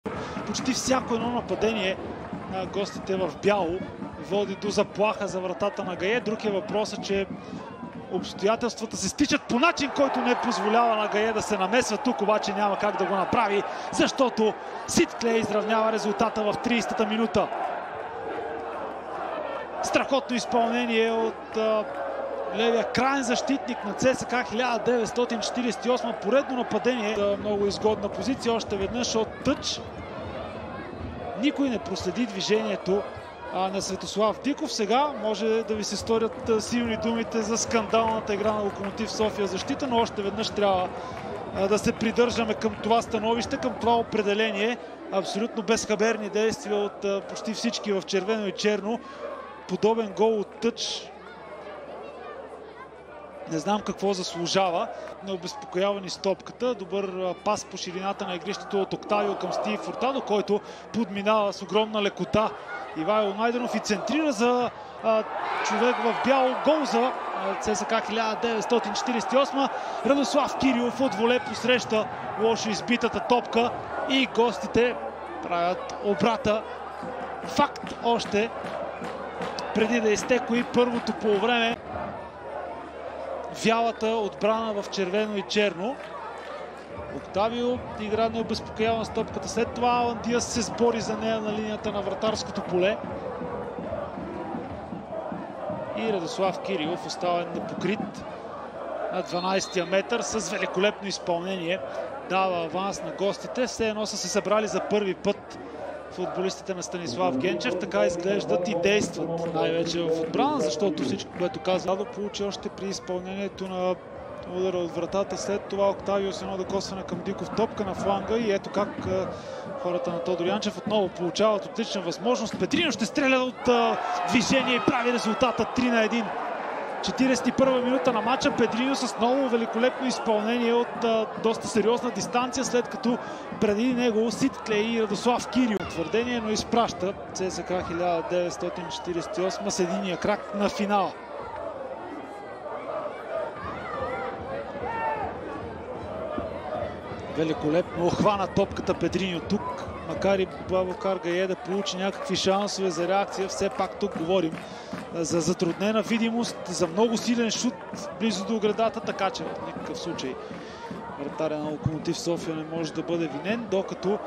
ЦСКА 1948 с три попадения срещу Локо София
ЦСКА 1948 с три попадения срещу Локо София в първия полуфинал за SESAME Купата на България по футбол.